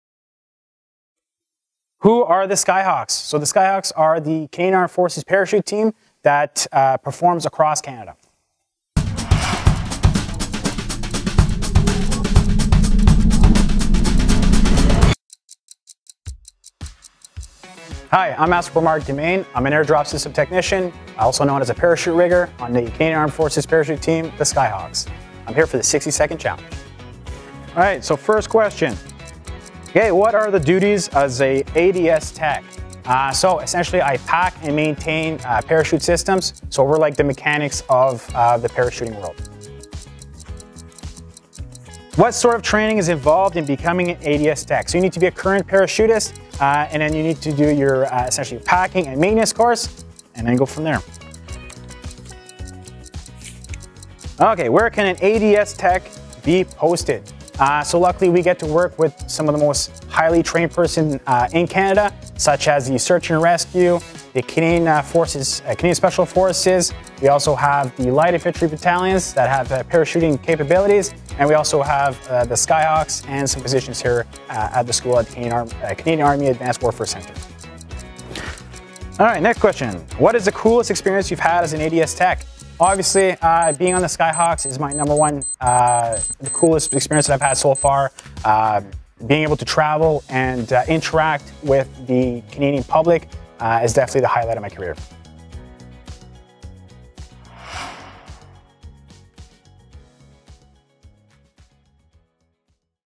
60 Secondes avec le Cplc Marc Dumaine, technicien de systèmes de largage
Ce n’est pas voler, c’est tomber avec du style! Voyez le Cplc Marc Dumaine répondre au plus grand nombre de questions possible au sujet de son rôle dans les SkyHawks durant le défi de 60Secondes.